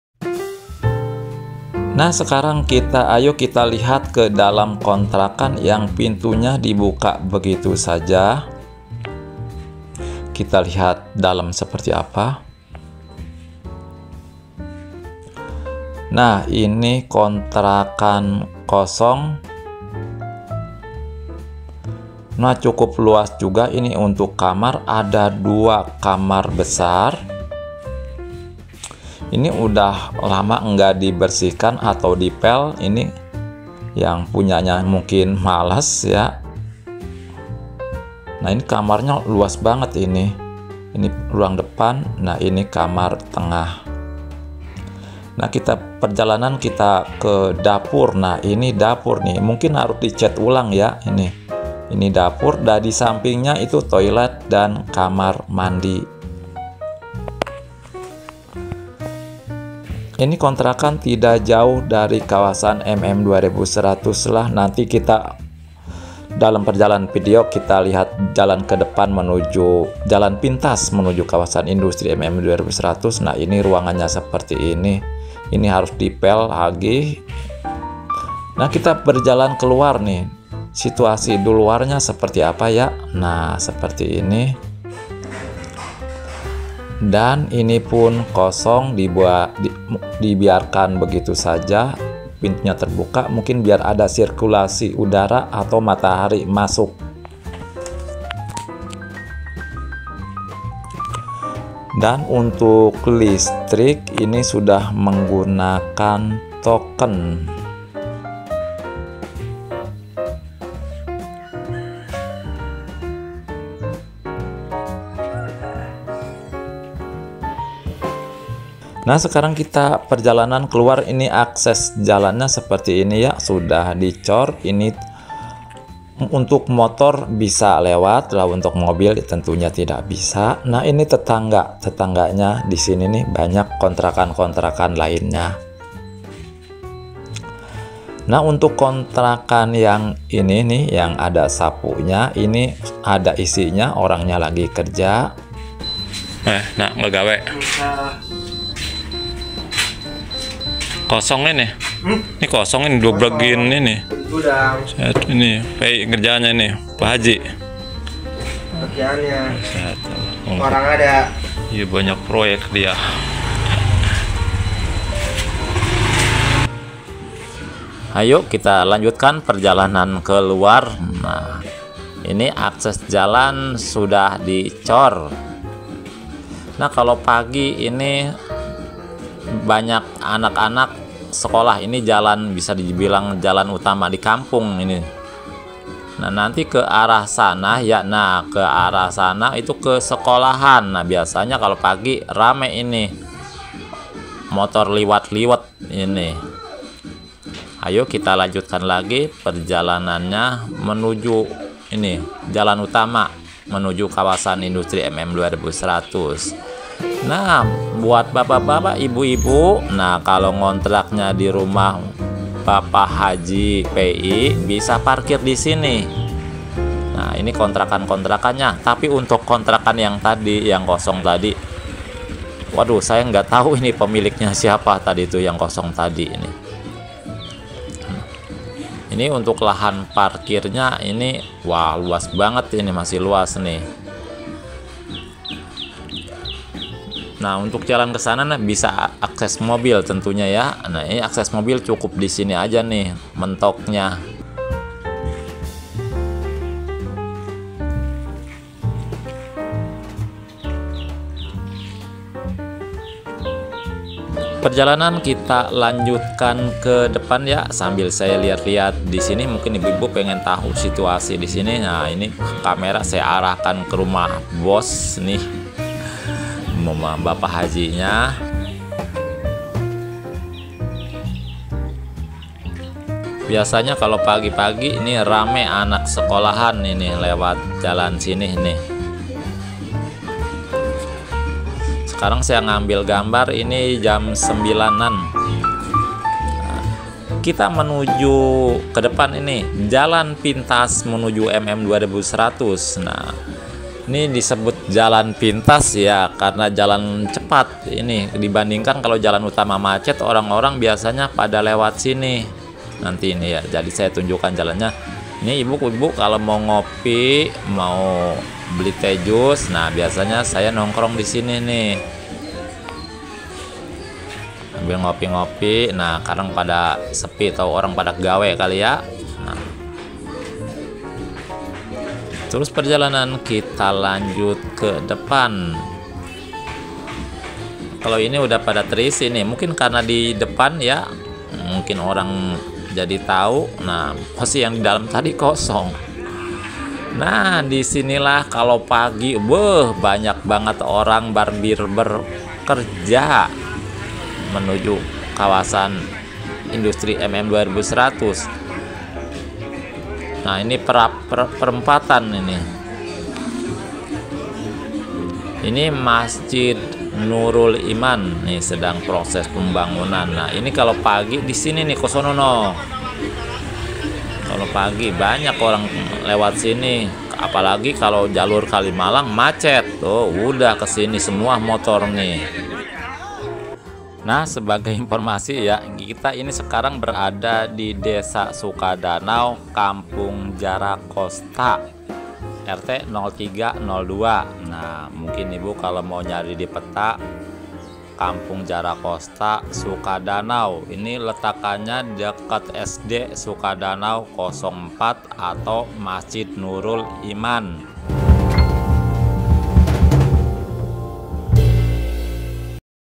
KONTRAKAN KOSONG CIKARANG JARANG DIKETAHUI
Kontrakan kosong Cikarang,dengan 2 kamar luas plus dapur dan kamar mandi tolilet di dalam .
Posisinya di dalam jadi jarang orang ketahui.
Lokasi kontrakan sudah saya jelaskan dalam video.